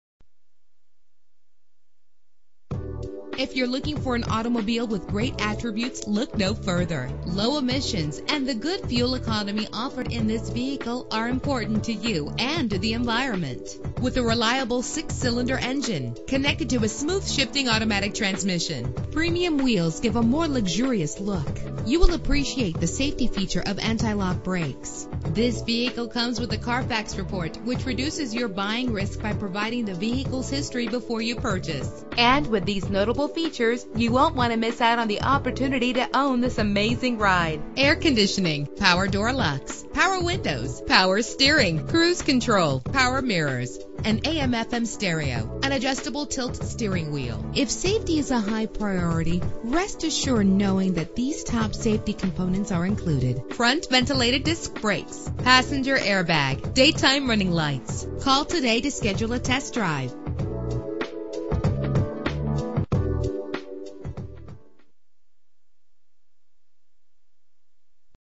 1998 Pontiac Grand Prix - Hillsboro OR
Year: 1998
Make: Pontiac
Model: Grand Prix
Trim: SE
Engine: 3.8 liter 6 cylinder 12 valve
Transmission: 4-Speed Automatic
Color: Green
Mileage: 64495
Address:
4151 S. E. Tualatin Valley Highway